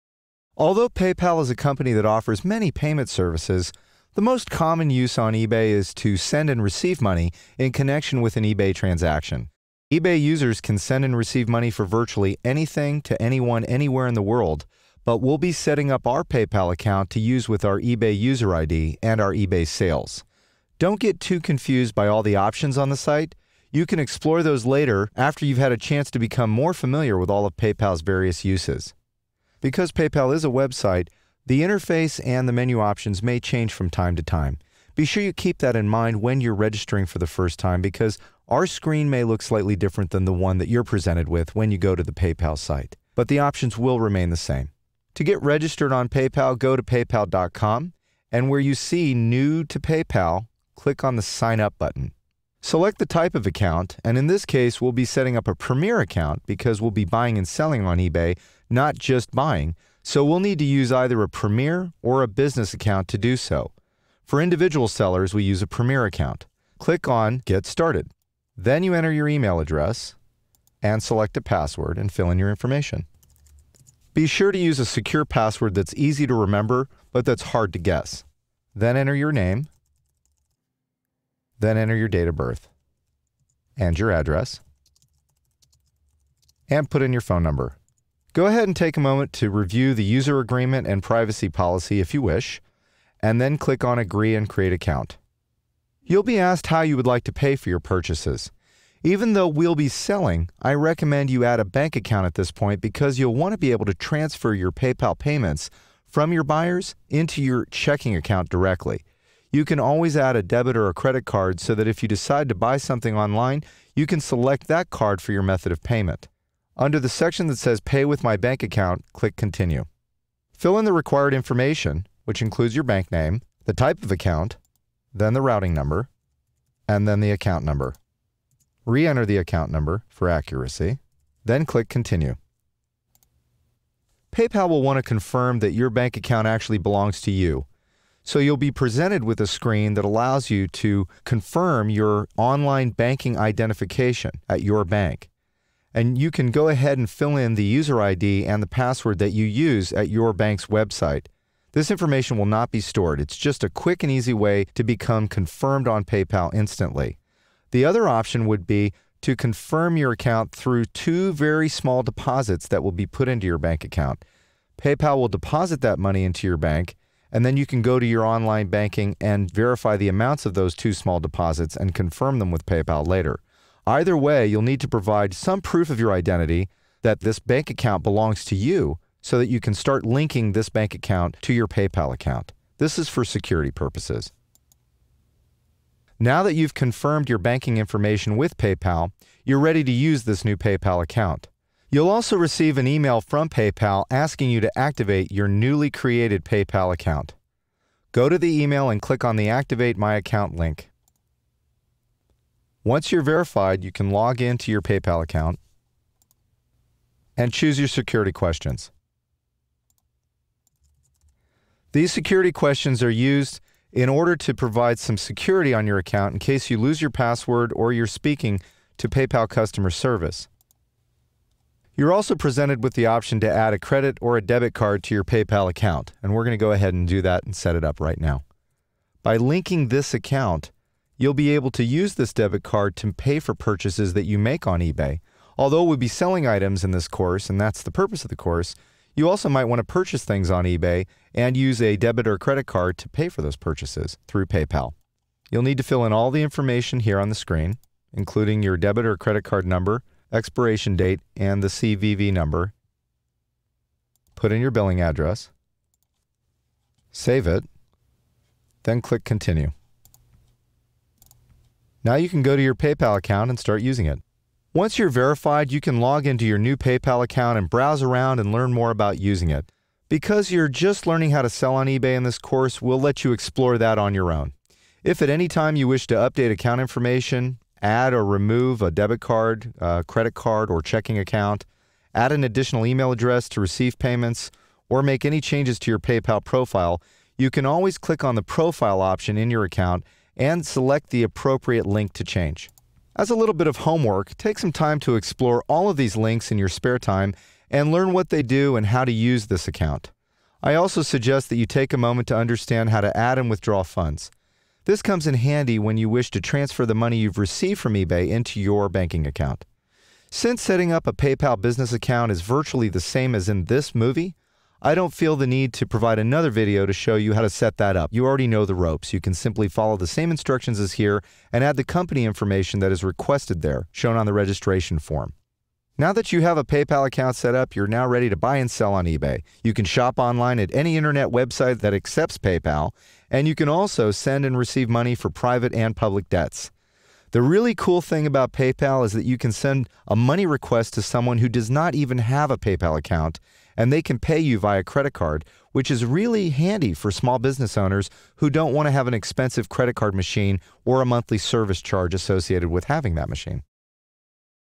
eBay For Beginners ( Creating an account on PayPal )
It's easy—simply log in to PayPal.com and select the Send Money tab. Click Change to select PayPal Credit as your payment method, then confirm your payment details.